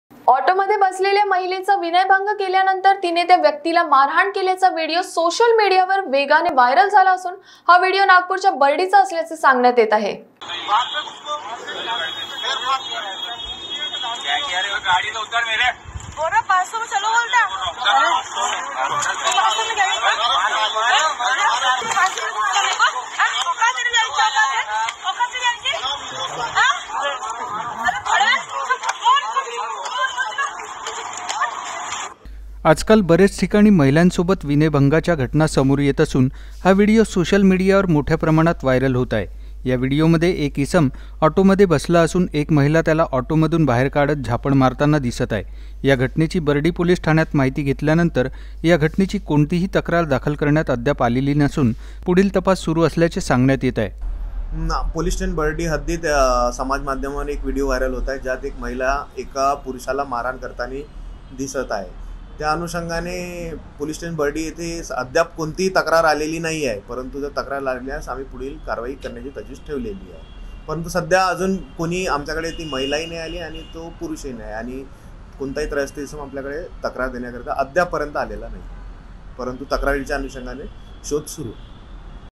विनयभंग केल्याप्रकरणी इसमाला केली मारहाण
ऑटोमध्ये बसलेल्या महिलेचा विनयभंग केल्यानंतर तिने त्या व्यक्तीला मारहाण केल्याचा व्हिडीओ सोशल मीडिया वर वेगाने व्हायरल झाला असून, हा व्हिडिओ नागपूरच्या बर्डीचा असल्याचे सांगण्यात येत आहे.